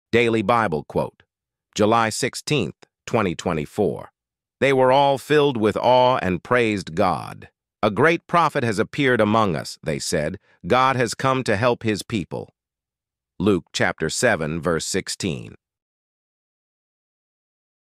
Luke 7:16 | Daily Bible Quote July 16, 2024
Bible quote of the day - July 16, 2024 | Luke 7:16
They were all filled with awe and praised God. "A great prophet has appeared among us," they said. "God has come to help his people." [NIV]
Then fear came upon all, and they glorified God, saying, “A great prophet has risen up among us”; and, “God has visited His people.” [NKJV]
Fear seized them all, and they glorified God, saying, “A great prophet has arisen among us!” and “God has visited his people!” [ESV]
Great fear swept the crowd, and they praised God, saying, “A mighty prophet has risen among us,” and “God has visited his people today.” [NLT]
Then fear came over everyone, and they glorified God, saying, “A great prophet has risen among us,” and “God has visited his people.” [CSB]